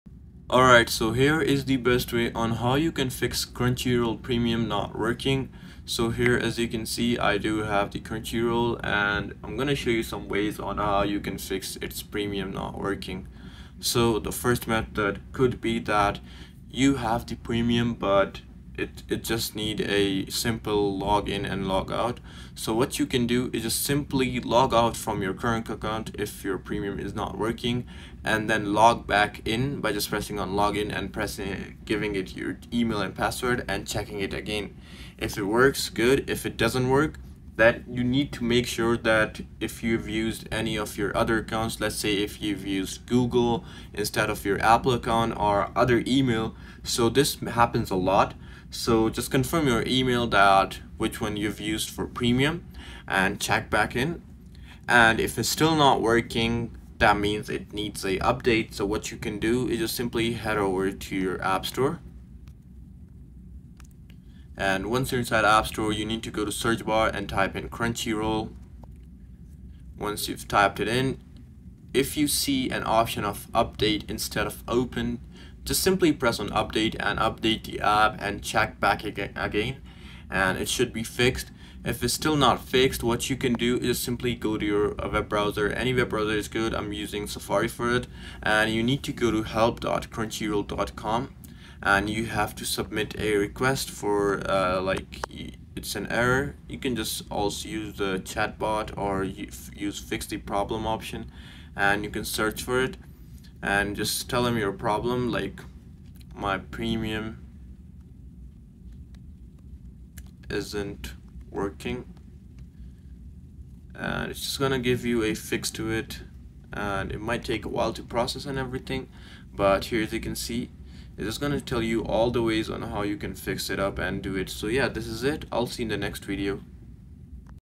How To Fix Crunchyroll Premium Not Working 2025 (STEP BY STEP)
How to Fix Crunchyroll Premium Not Working 2025 (Step-by-Step Guide)

In this quick tutorial, learn how to fix Crunchyroll Premium issues in 2025 — whether your subscription isn’t loading, videos won’t play, or premium features are missing. Follow these step-by-step solutions to get Crunchyroll working again fast!

✅ Works on Android, iOS, Smart TVs & PC
⚡ Fix login, payment & playback problems
🔧 Updated solutions for 2025
💯 100% safe and beginner-friendly